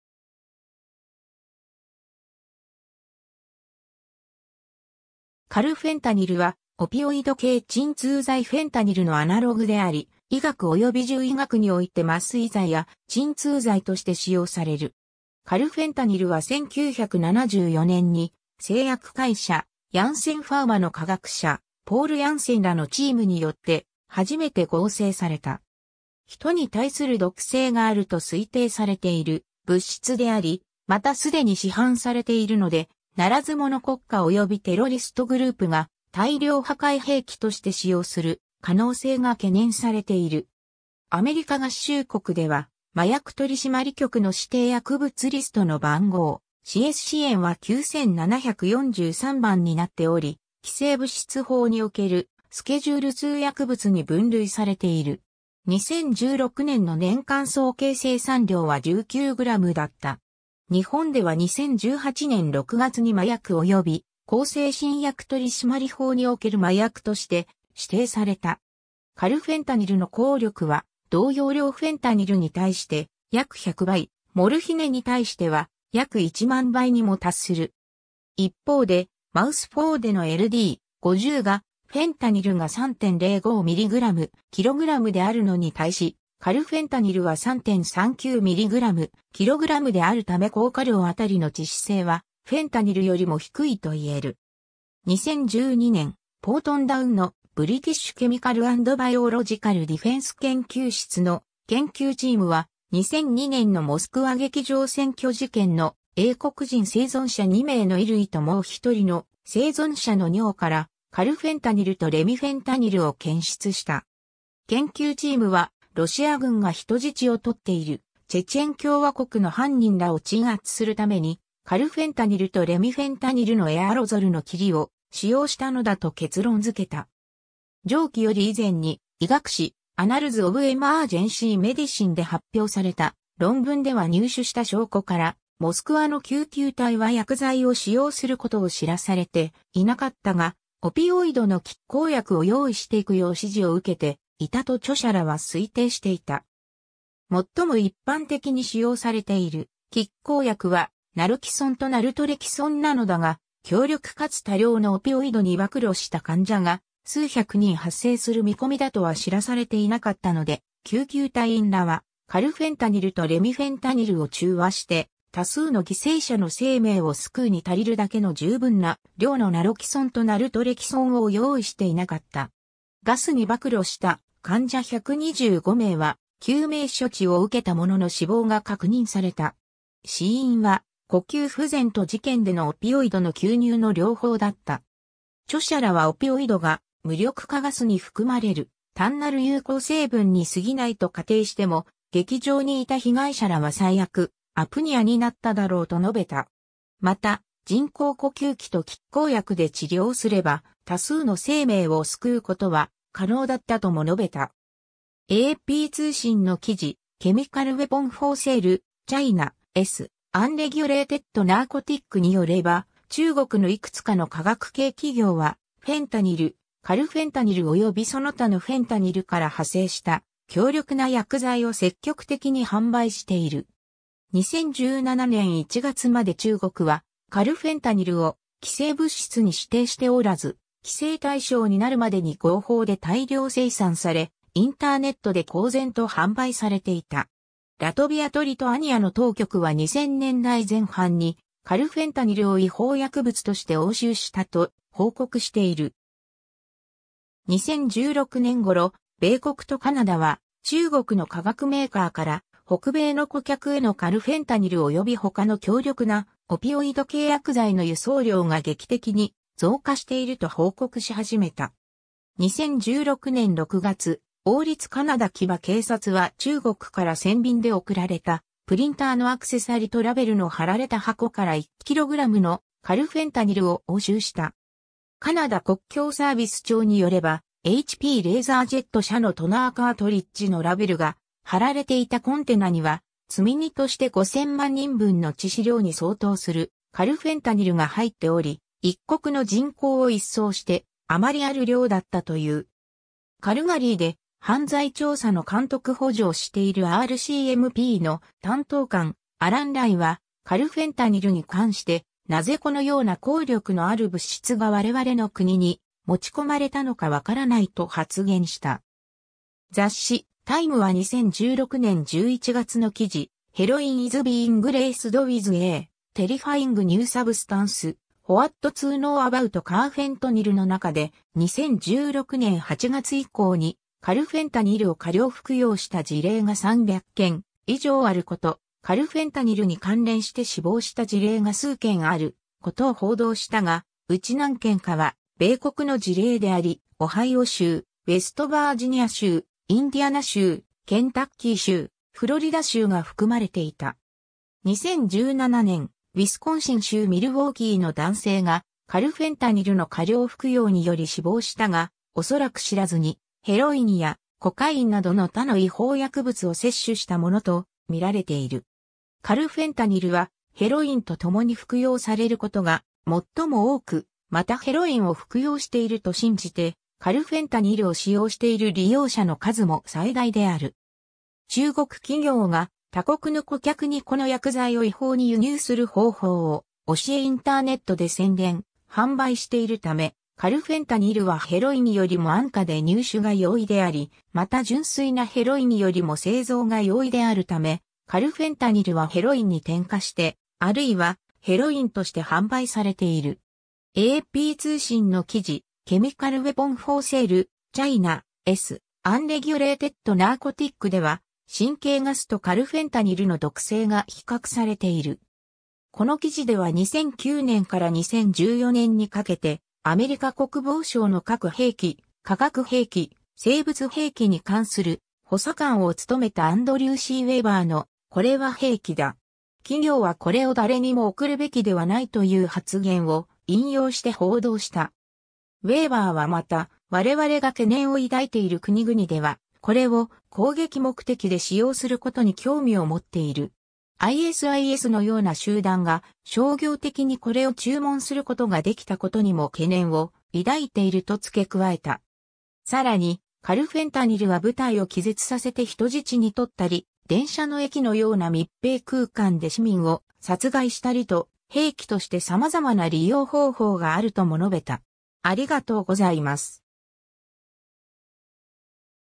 カルフェンタニル
カルフェンタニル（英：carfentanil、別名：4-カルボメトキシフェンタニル）はオピオイド系鎮痛剤フェンタニルのアナログであり、医学および獣医学において麻酔剤や鎮痛剤として使用される。
カルフェンタニルは1974年に製薬会社ヤンセン ファーマの化学者ポール・ヤンセンらのチームによって初めて合成された。
ヒトに対する毒性があると推定されている物質であり、また既に市販されているので、ならず者国家およびテロリストグループが大量破壊兵器として使用する可能性が懸念されている。
アメリカ合衆国では麻薬取締局の指定薬物リストの番号ACSCNは9743番になっており、規制物質法におけるスケジュールII薬物に分類されている。
2016年の年間総計生産量は19グラムだった。
日本では2018年6月に麻薬及び向精神薬取締法における麻薬として指定された。
 カルフェンタニルの効力は同用量フェンタニルに対して約100倍、モルヒネに対しては約10,000倍にも達する。
一方で、マウスIVでのLD/50がフェンタニルが3.05 mg/kgであるのに対し、カルフェンタニルは3.39 mg/kgであるため効果量あたりの致死性はフェンタニルよりも低いといえる。
 2012年、ポートンダウンのBritish chemical and biological defense（直訳：英国化学生物防御）研究室の研究チームは、2002年のモスクワ劇場占拠事件の英国人生存者2名の衣類ともう1人の生存者の尿からカルフェンタニルとレミフェンタニルを検出した。
研究チームはロシア軍が人質をとっているチェチェン共和国の犯人らを鎮圧するためにカルフェンタニルとレミフェンタニルのエアロゾルの霧を使用したのだと結論づけた。
 上記より以前に医学誌『Annals of Emergency Medicine』で発表された論文では入手した証拠から、モスクワの救急隊は薬剤を使用することを知らされていなかったがオピオイドの拮抗薬を用意していくよう指示を受けていたと著者らは推定していた。
最も一般的に使用されている拮抗薬はナロキソンとナルトレキソンなのだが、強力かつ多量のオピオイドに曝露した患者が数百人発生する見込みだとは知らされていなかったので、救急隊員らはカルフェンタニルとレミフェンタニルを中和して多数の犠牲者の生命を救うに足りるだけの十分な量のナロキソンとナルトレキソンを用意していなかった。
ガスに曝露した患者125名は救命処置を受けたものの死亡が確認された。
死因は呼吸不全と事件でのオピオイドの吸入の両方だった。
著者らはオピオイドが無力化ガスに含まれる単なる有効成分にすぎないと仮定しても、劇場にいた被害者らは最悪Apnea（呼吸停止）になっただろうと述べた。
また、人工呼吸器と拮抗薬で治療すれば多数の生命を救うことは可能だったとも述べた。
 AP通信の記事『Chemical weapon for sale: China's unregulated narcotic』（訳：市販されている化学兵器：中国の非規制麻薬）によれば、中国のいくつかの化学系企業はフェンタニル、カルフェンタニルおよびその他のフェンタニルから派生した強力な薬剤を積極的に販売している 。
2017年1月まで中国はカルフェンタニルを規制物質に指定しておらず、規制対象になるまでに合法で大量生産され、インターネットで公然と販売されていた。
 ラトビアとリトアニアの当局は2000年代前半にカルフェンタニルを違法薬物として押収したと報告している。
2016年頃、米国とカナダは中国の化学メーカーから北米の顧客へのカルフェンタニルおよび他の強力なオピオイド系薬剤の輸送量が劇的に増加していると報告し始めた。
2016年6月、王立カナダ騎馬警察 (RCMP)は中国から船便で送られた「プリンターのアクセサリ」とラベルの貼られた箱から1キログラムのカルフェンタニルを押収した。
カナダ国境サービス庁によればHP LaserJet社のトナーカートリッジのラベルが貼られていたコンテナには、積荷として5000万人分の致死量に相当するカルフェンタニルが入っており、1国の人口を一掃して余りある量だったという。
カルガリーで犯罪調査の監督補助をしているRCMPの担当官アラン・ライ (Allan Lai)は、「カルフェンタニルに関して、なぜこのような効力のある物質が我々の国に持ち込まれ...